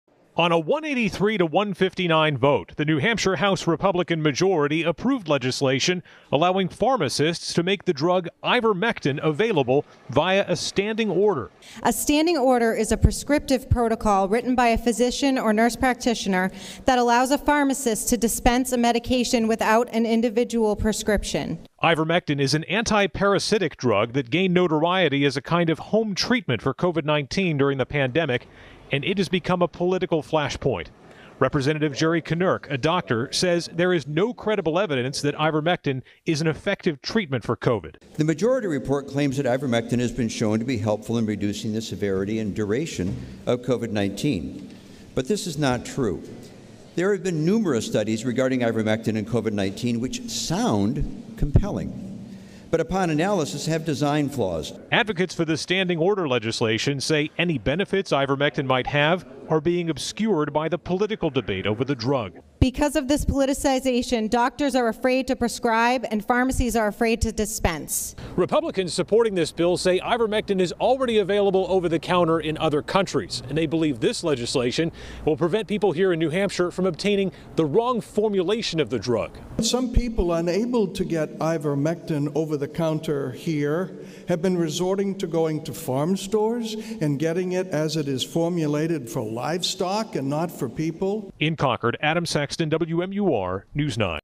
GOP state reps. pass bill to make ivermectin available via standing order at pharmacies
On a 183-159 vote, the House approved legislation allowing pharmacists to make ivermectin available via standing order.